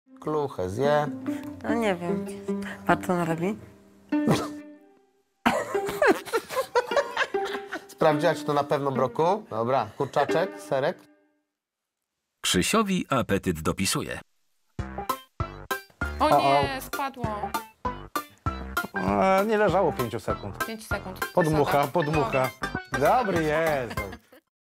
BROKUŁEK 🥦 | Jak dobrze znacie swoje dzieci?
Tylko rodzice wiedzą do czego są zdolne ich dzieci… ale czy znają je na wylot i potrafią przewidzieć co zrobią w konkretnych sytuacjach?